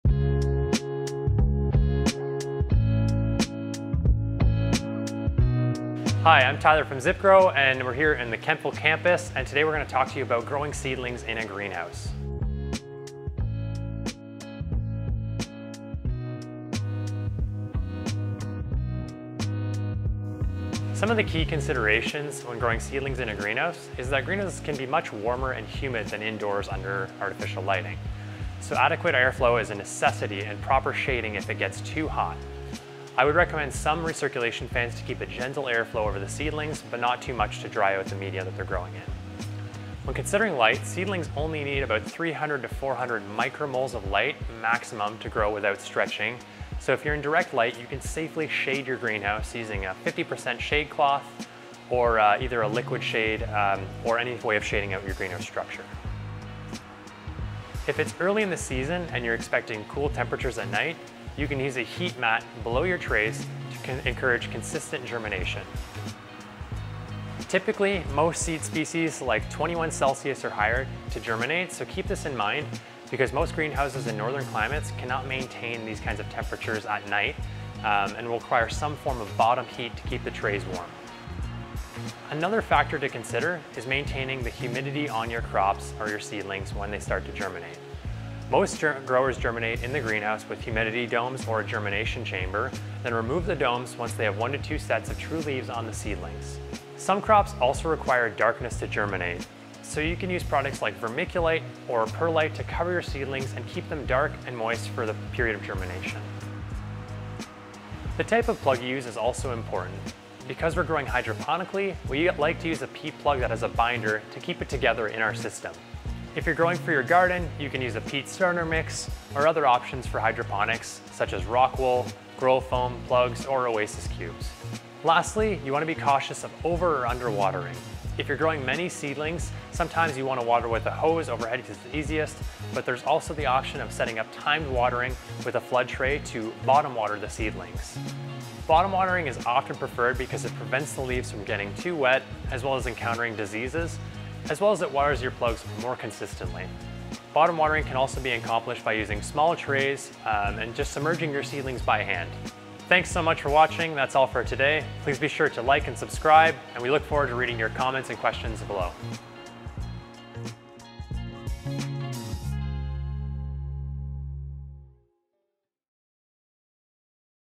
Growing seedlings in a greenhouse
In this video, Tyler explains key considerations when growing seedlings in a greenhouse setting. Seedlings in a greenhouse will experience different conditions than indoors under artificial lighting and controlled temperatures. We have some tips for you that will help optimize growing conditions for happy healthy plants.

This video includes:
Air flow
Light & Shade
Heat
Humidity
Growing media
Watering